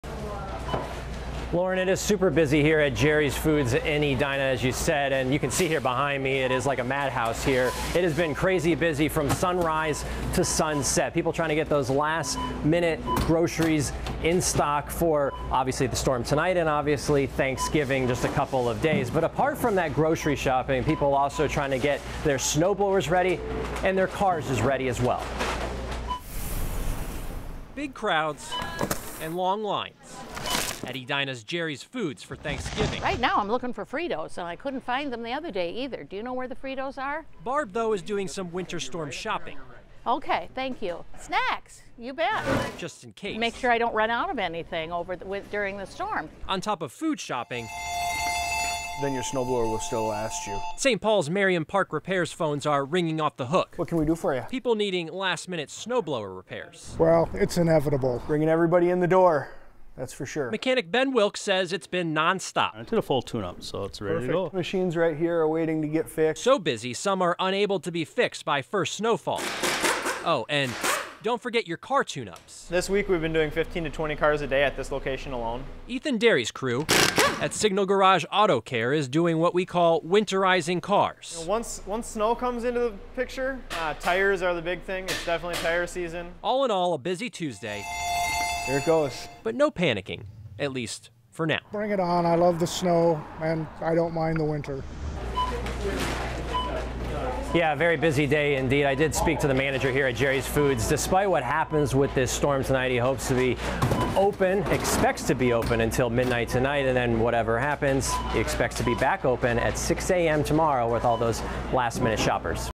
Busy day in preparation for winter storm and Thanksgiving
People were out getting food, fixing snowblowers, and getting their cars ready.